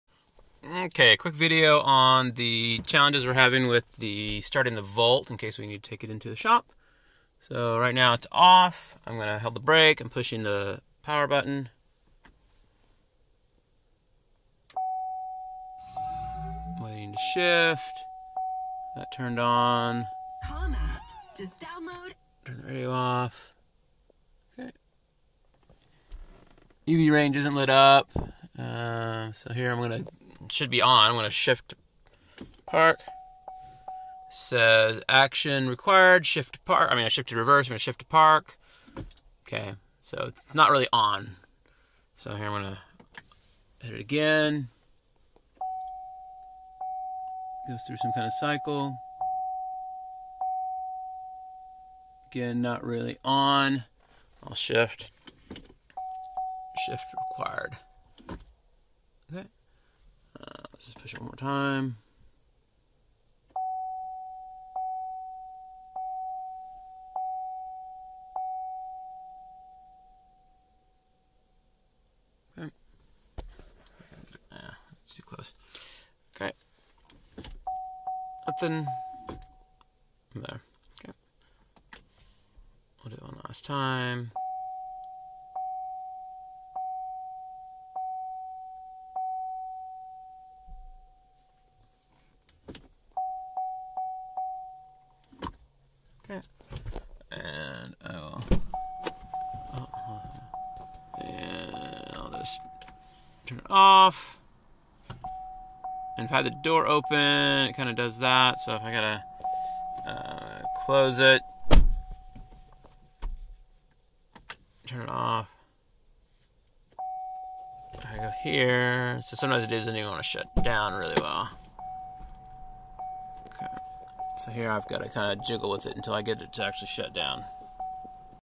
2017 Chevy Volt Starting Issue - Replaced BECM
A problem I am having with my 2017 Chevy volt. It fails to go into ready mode when starting.

For those of you looking for a solution, turned out to be the BECM (Battery Engine Control Module). We were able to get it replaced by the dealer for free--it is a known problem.